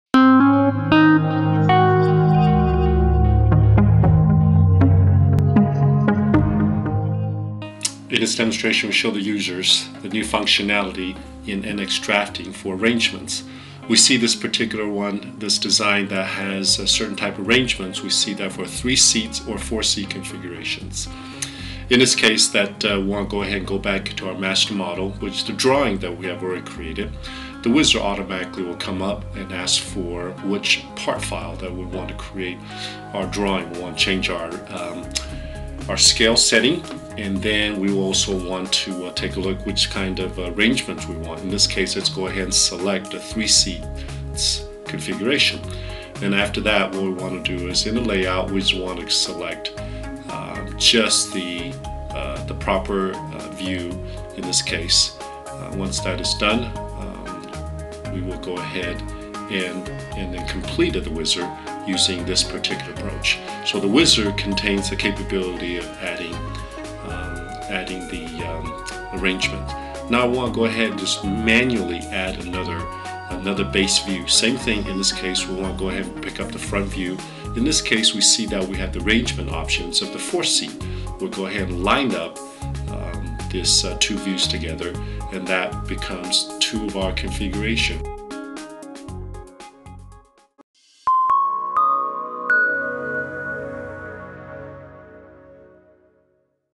NX 11 - Drawing Views with Embedded Arrangements with NX CAD
This video demonstrates the new drafting functionality for arrangements in NX 11.